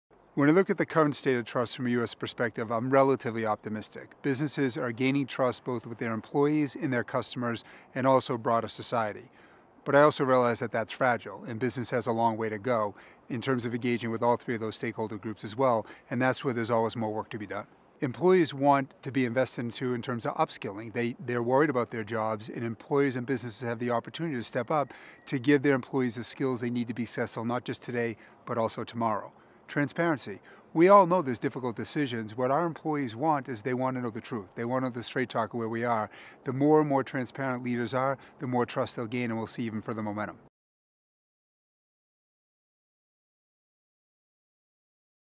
Tim Ryan on the State of Trust in the U.S.
Tim Ryan (Chairman and Senior Partner, PwC U.S.) shares his thoughts on the state of trust in business from a U.S. perspective

View the TrustBarometer findings for more insight